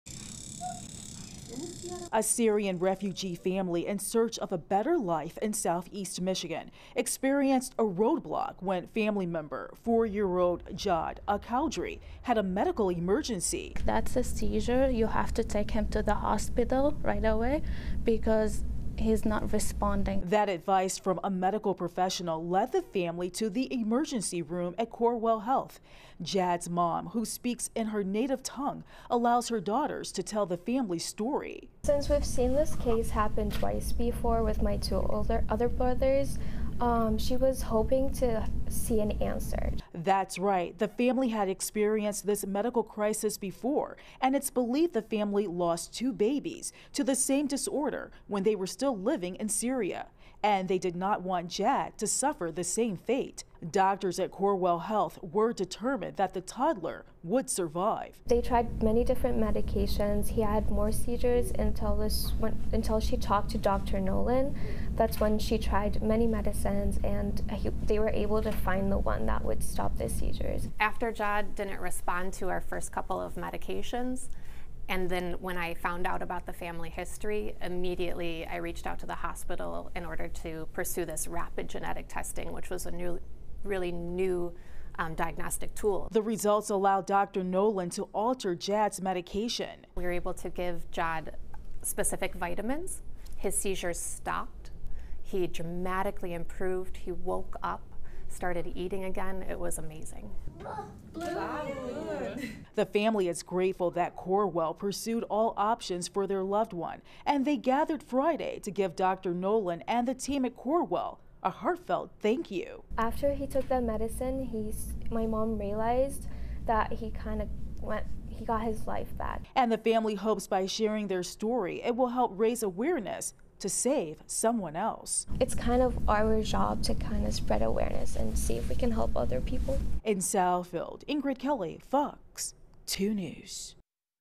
Life-saving treatment for toddler's seizures: family's heartfelt thanks to Corewell Health
A 4-year-old boy is doing well and not experiencing seizures after doctors found medications that would help. The boy has a genetic condition that his family lost two other children to.